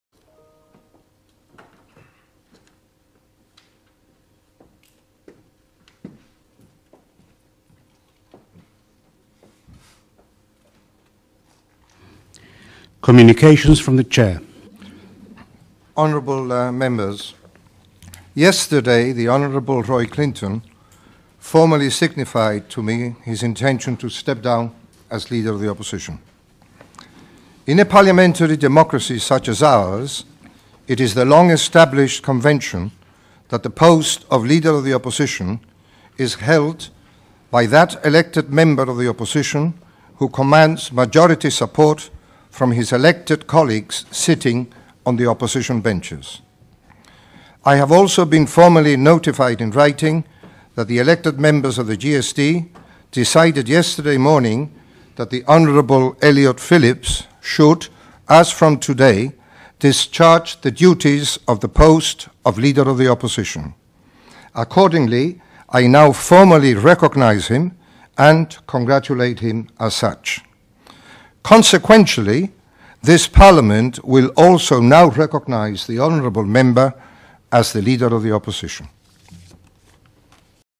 Speaker Adolfo Canepa on Elliott Phillips as Leader of the Opposition
Images courtesy of Gibraltar Parliament